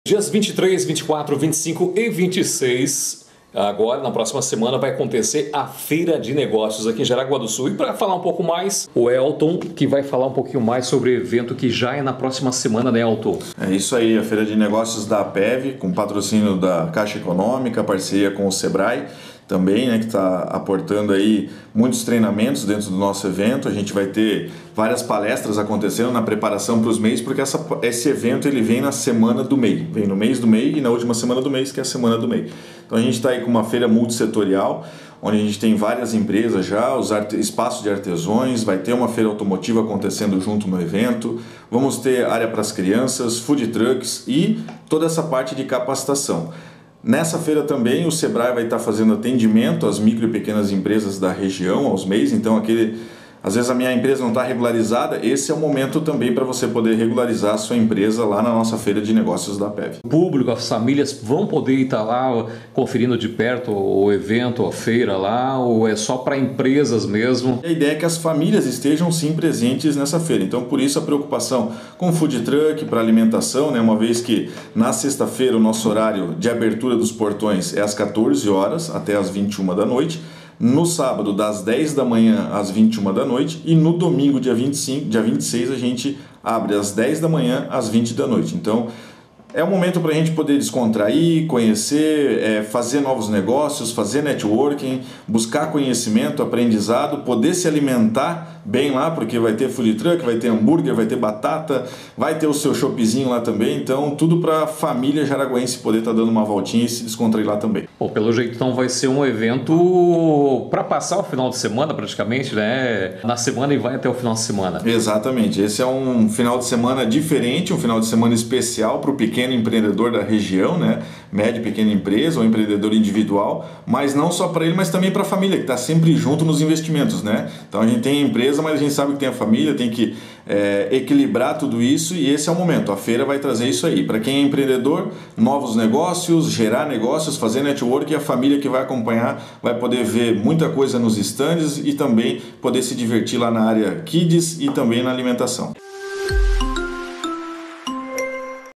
FEIRA DE NEGÓCIOS APEVI DE 23 à 26 de MAIO
No próximo final de semana acontece a Feira de Negócios em Jaraguá do Sul, o evento vai acontecer no Pavilhão de Eventos da Cidade. Reunindo muitas atrações, como feira automotiva, artesanatos, pequenas e micro empresas expondo seu trabalho e serviço.